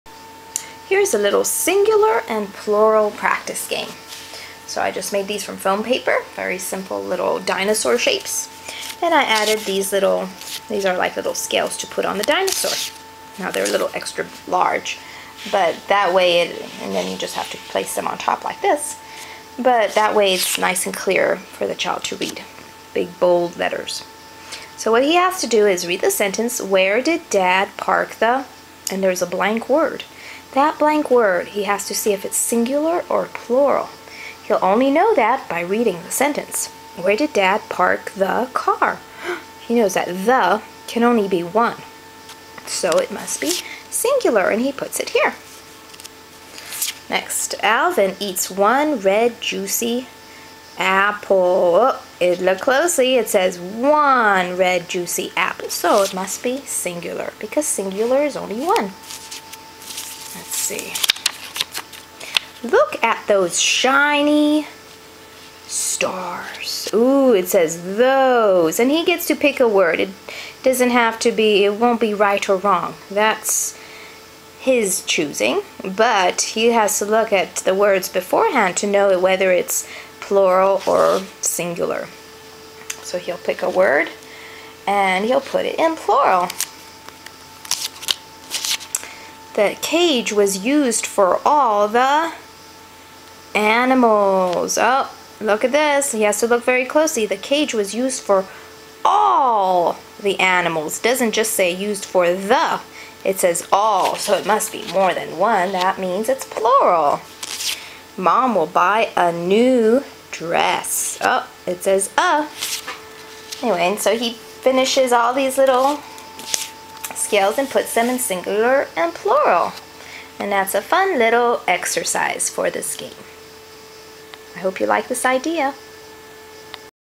Grade 1 - Language, spelling: Singular and Plural dinosaurs foam game.
Educational game for kids practicing with singular and plural nouns.
Including sentence structure and filling the blanks.
Paying attention to surrounding words or articles to determine the singular or plural word.
Theme: dinosaurs, green, color, animals

Fun ideas and activities for primary school learning.